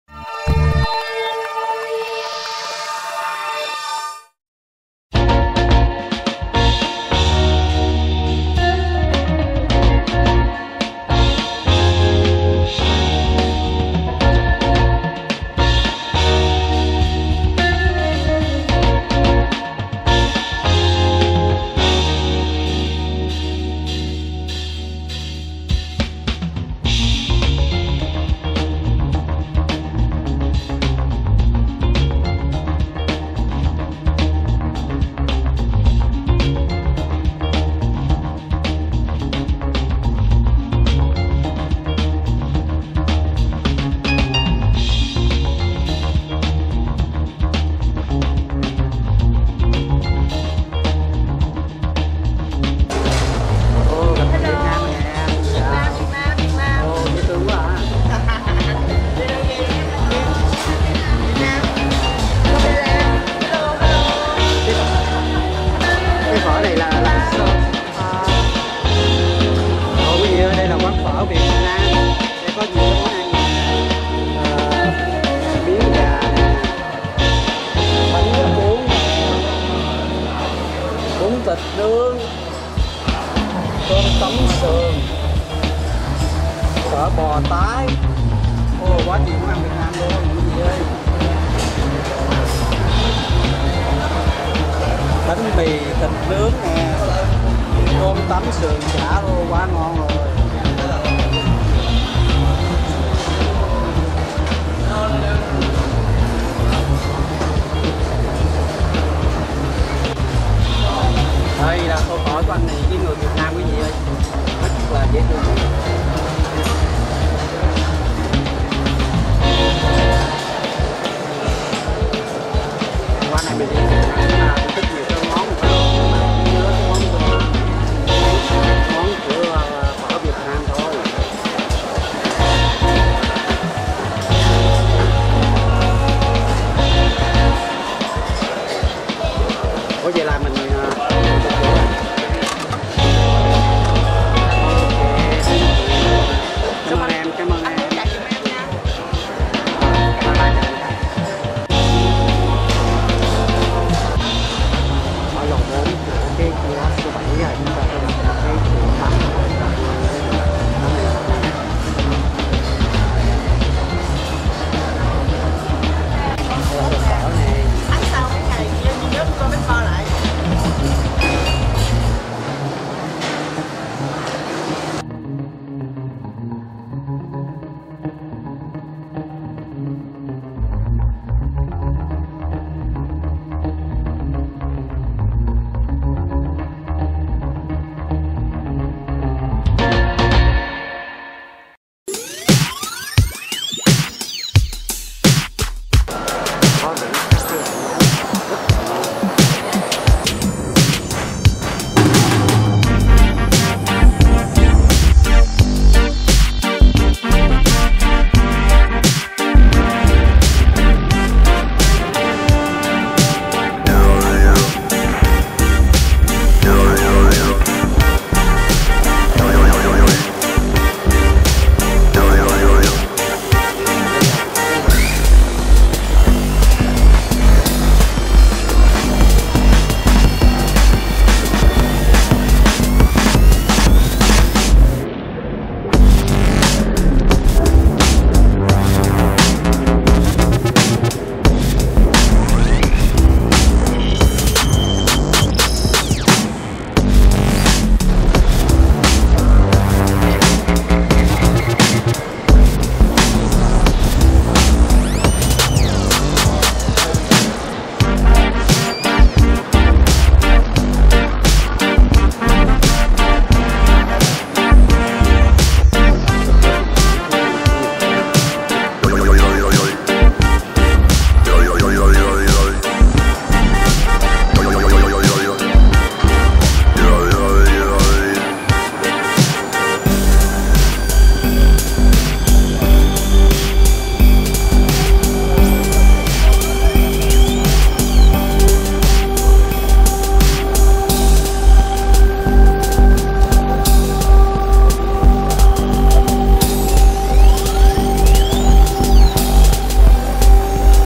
KHÁM PHÁ KHU ẨM THỰC SIÊU THỊ BIG C THỦ ĐÔ BANGKOK THÁI LAN
KHU ẨM THỰC RẤT RỘNG LỚN TRÊN TẦNG 4 CỦA SIÊU THỊ BIG C, CÓ NHIỀU KHU VỰC CHO NHIỀU HÀNG QUÁN CỦA NHIỀU KHẨU VỊ CỦA CÁC NƯỚC, MÌNH ĐẶC BIỆT THÍCH 1 KHU VỰC CỦA 1 VỊ CHỦ NHÂN LÀ NGƯỜI VIỆT NAM KINH DOANH, CÓ RẤT NHIỀU MÓN ĂN THÂN THIẾT QUEN THUỘC CỦA VIỆT NAM... THÌ LẠI CHỌN MÓN PHỞ TRÊN ĐẤT THÁI...!